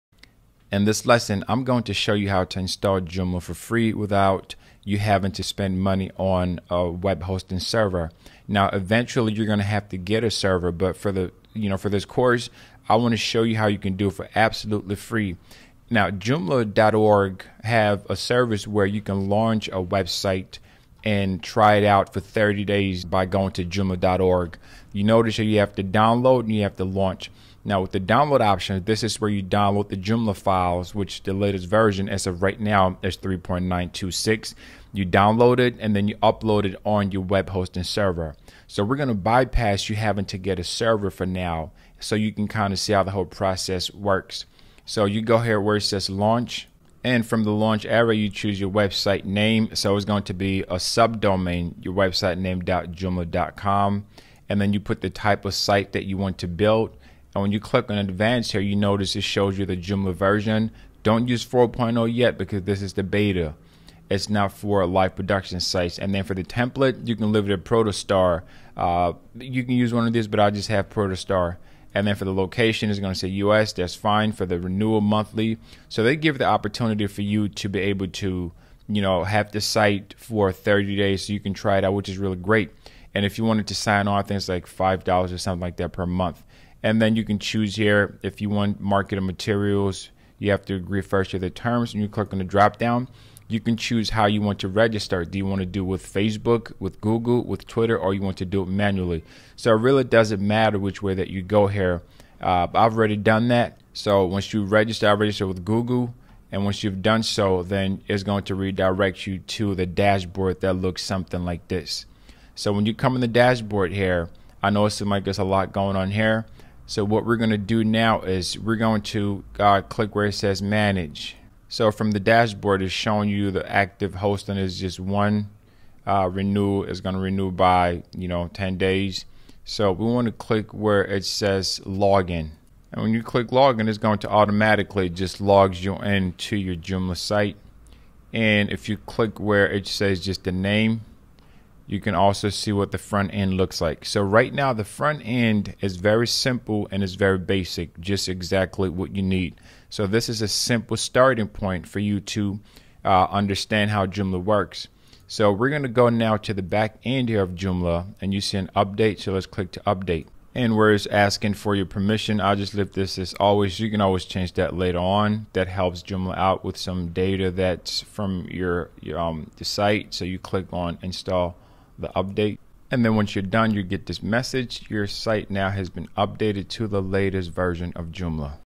Free Web Hosting for Joomla Website in 2021 - Fast Setup - Instant Access - Safe & Secure
Did you know that you can get FREE web hosting for your Joomla website. This is a great service provided by Joomla that allows you to build a Joomla website for free. Setup is fast and easy.

Get live one-on-one professional Joomla website design training...no coding experience required